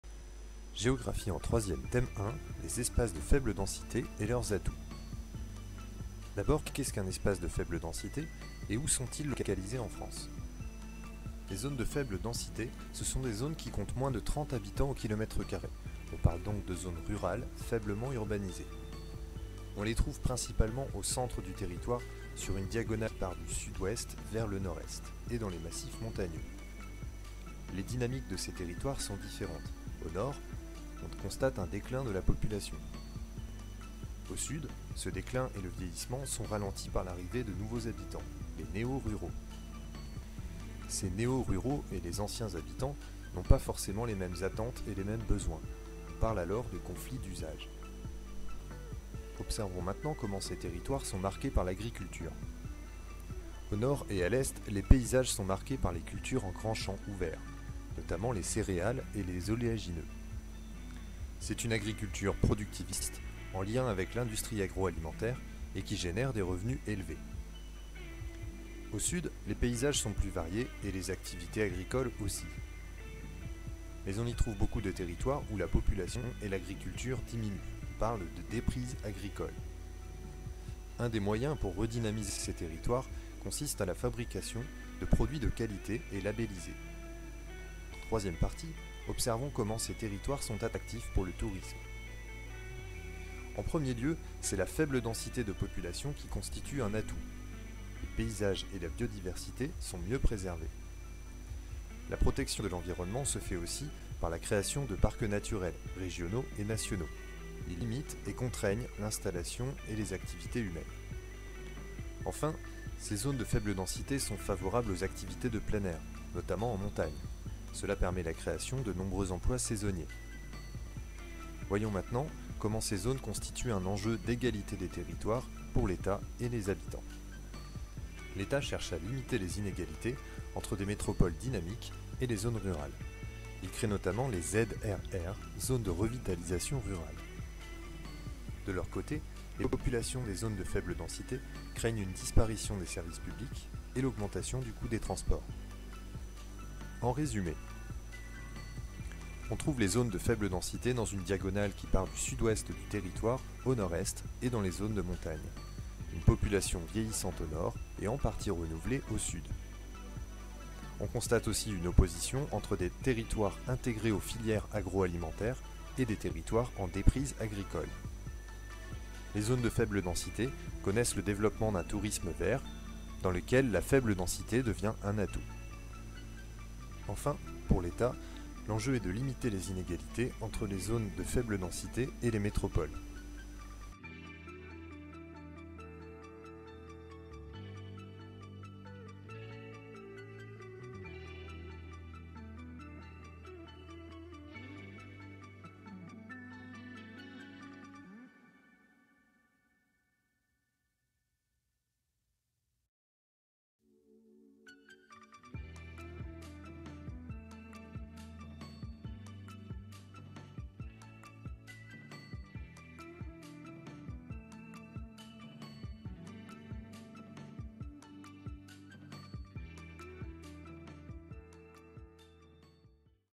Les espaces de faible densité et leurs atouts
Quels sont les atouts des espaces de faible densité ? Comment l'agriculture marque leurs paysages ? Quelles inégalités vis à vis des métropoles ?